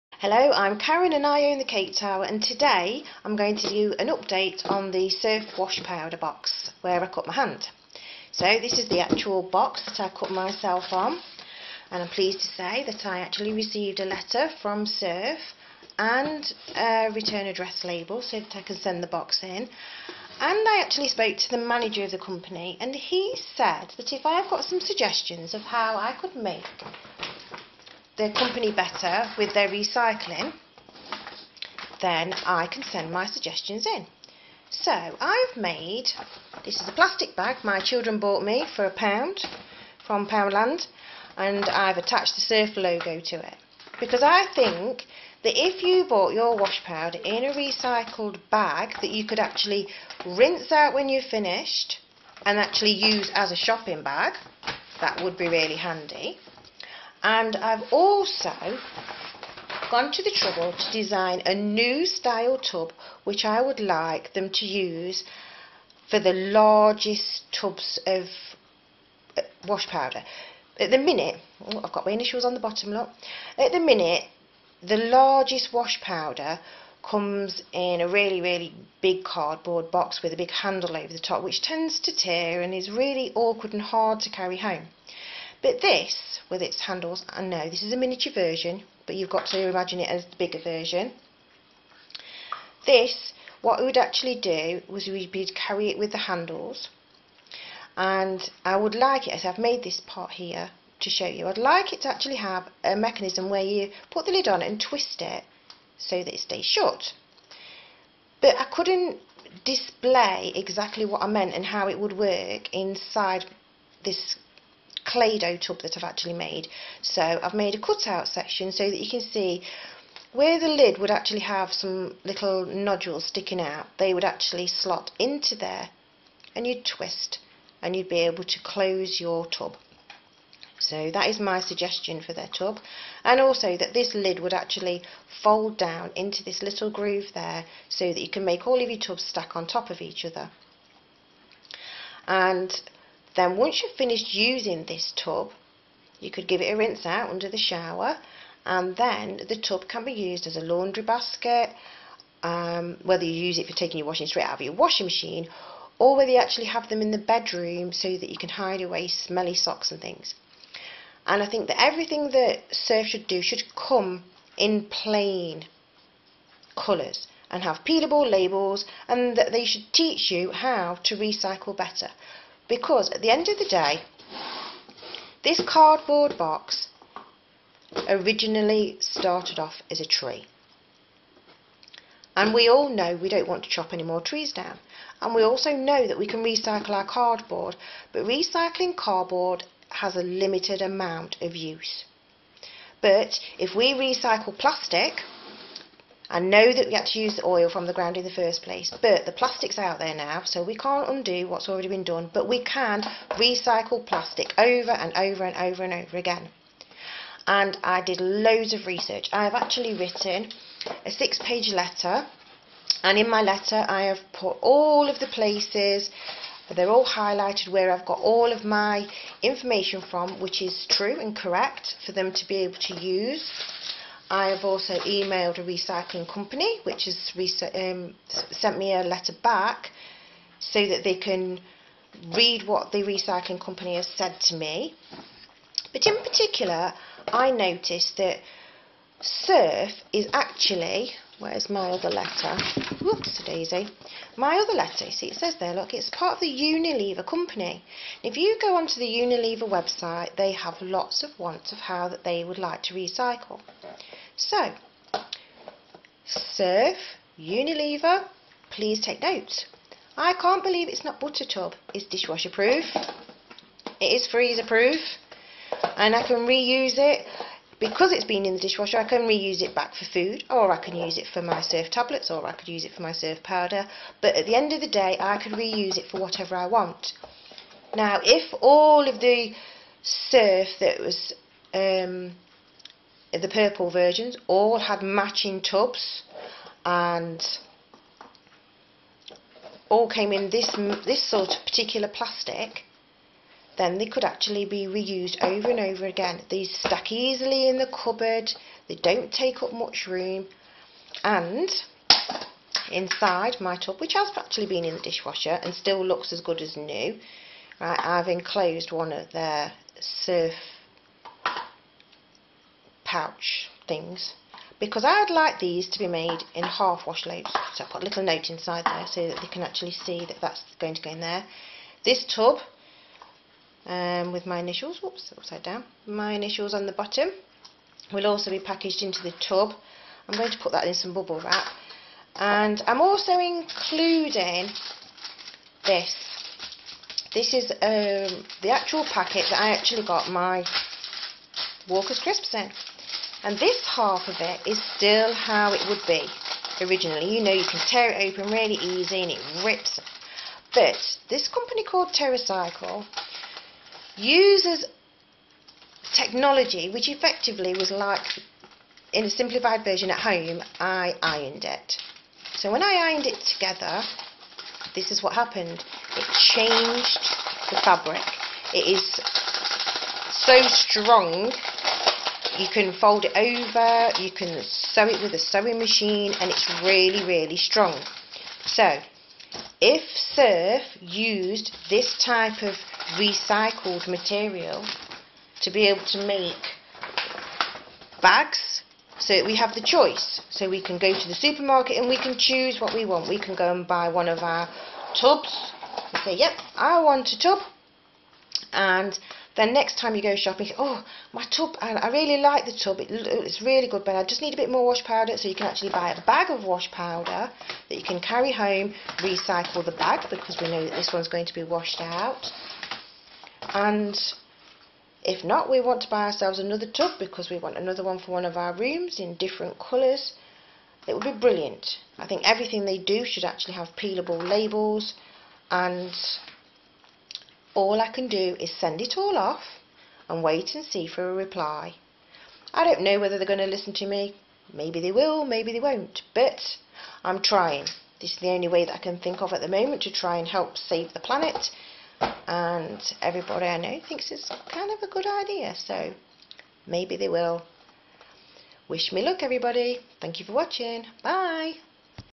Surf update and my new reusable tub design by The Cake Tower
This is my second video for Surf. I have now sent them my box back that I cut my hand on and my prototype for a new tub, plus some other ideas how to recycle and re-use their packaging. Surf and I can't believe it's not butter are both made by Unilever. Will they use my ideas???
Thank you to everyone for watching my videos